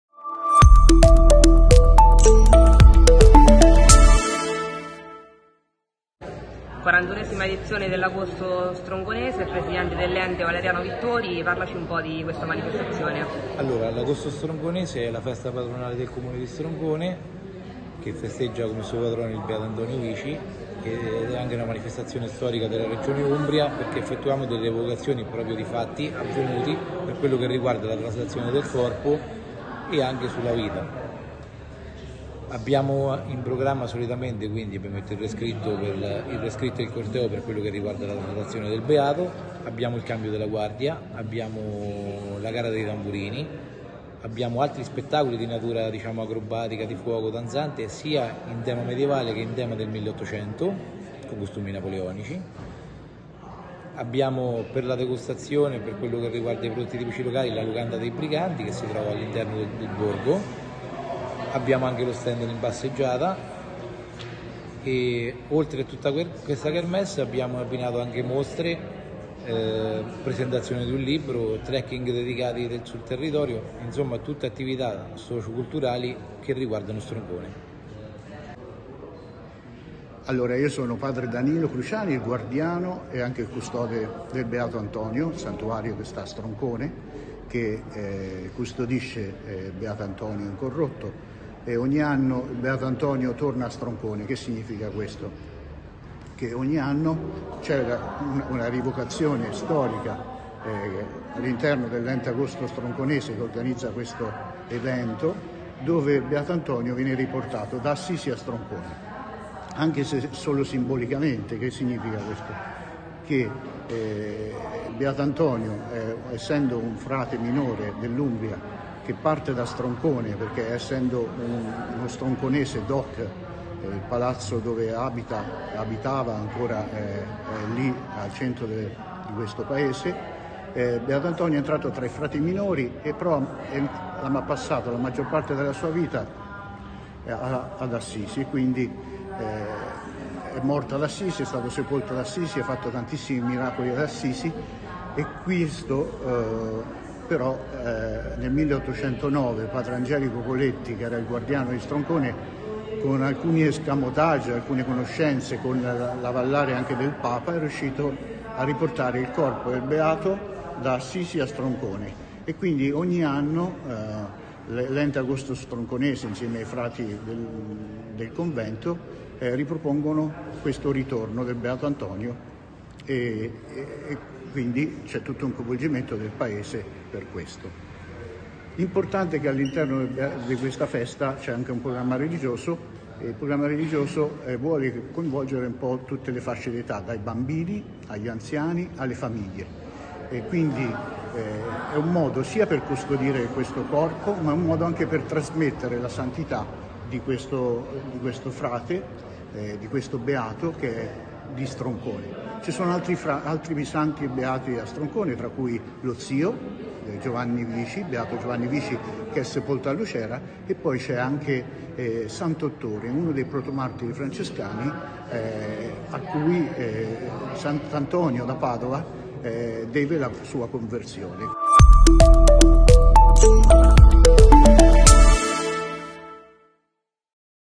Agosto Stronconese al via il 18 del mese: le voci dei protagonisti
Presentata nella sala consiliare della Provincia di Terni la 41esima edizione